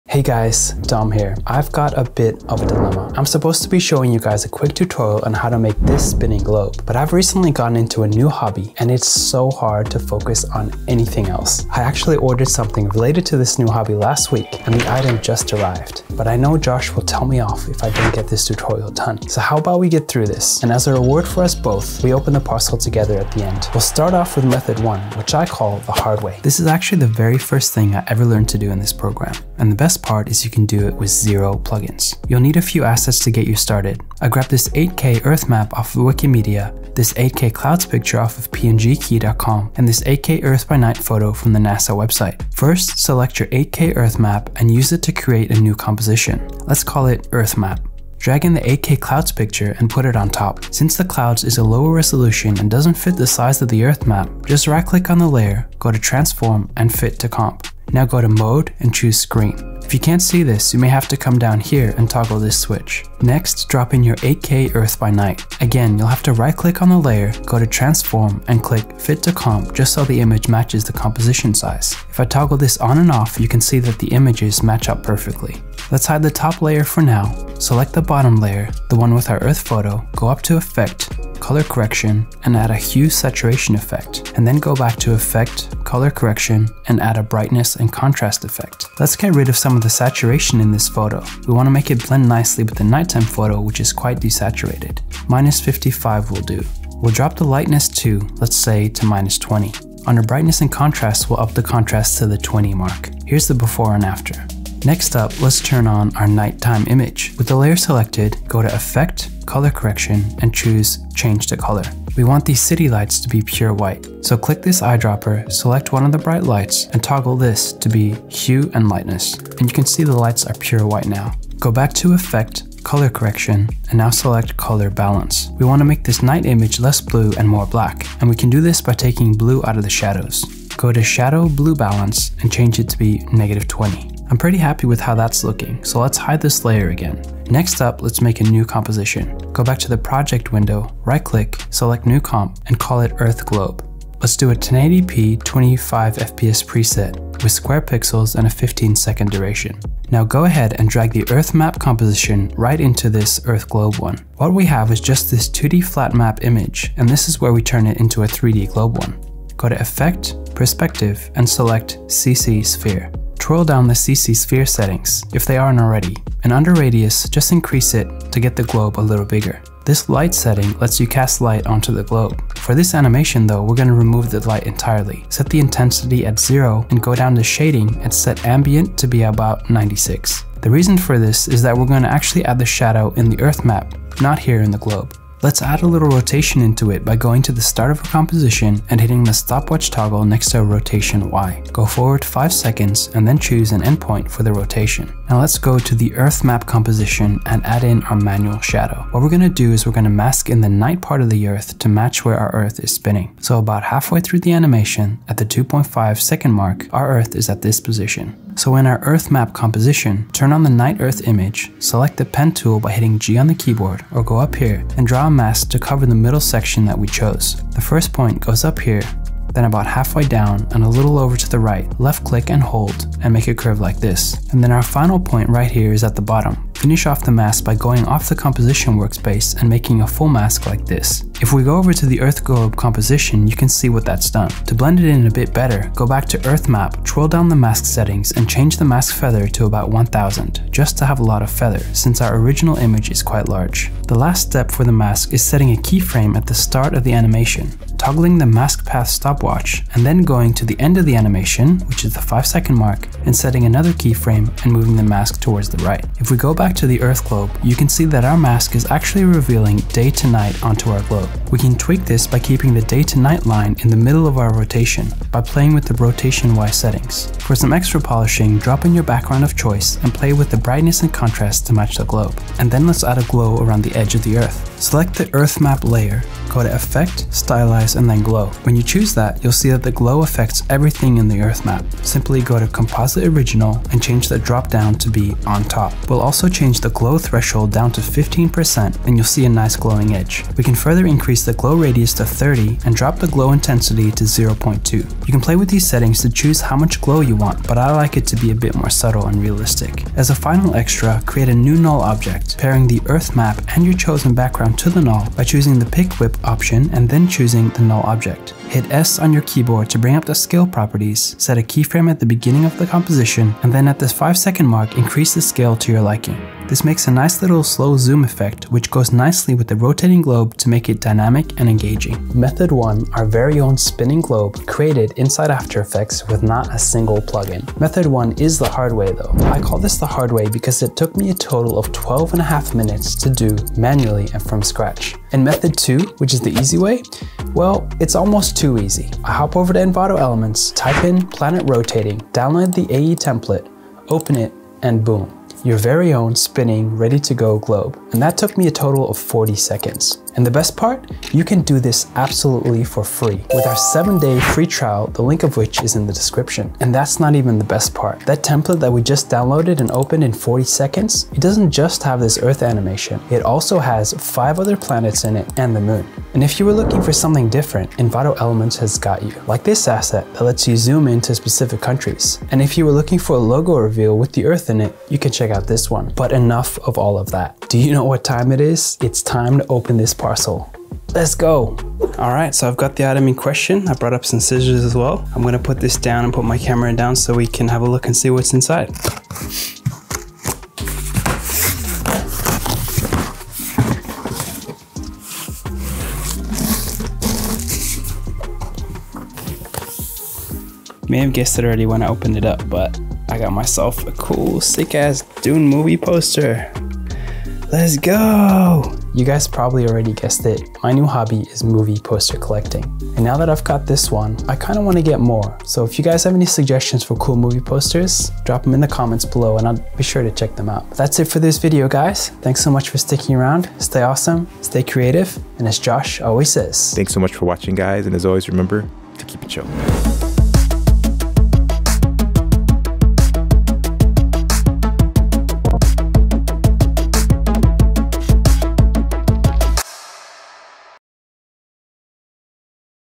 Spinning Globe in After Effects | Beginners Tutorial
In this After Effects beginner tutorial, Dom is going to show you how turn an image of the earth into a spinning planet in space.